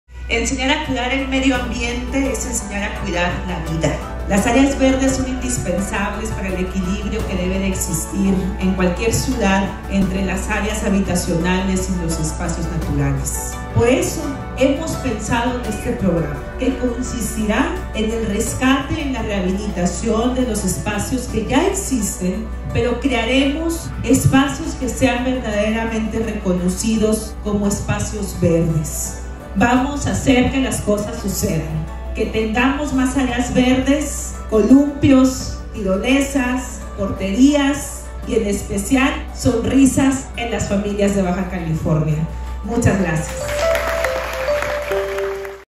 CON EL CORAZÓN POR MÁS PARQUES | Programa para el desarrollo de Áreas Verdes Urbanas
El Proyecto “Con el Corazón Por Más Parques”, permitirá transformar nuestro entorno con nuevas Áreas Verdes Urbanas, dando nuevos pulmones al estado.
Con esto aseguramos la sana convivencia en un espacio donde las familias puedan reunirse de nuevo y disfrutar de un paisaje que los haga sentir orgullosos de su estado.
Comenzamos en la Comunidad de Natura, en ciudad de Tijuana, sin embargo, replicaremos este proyecto por todos los municipios de Baja California.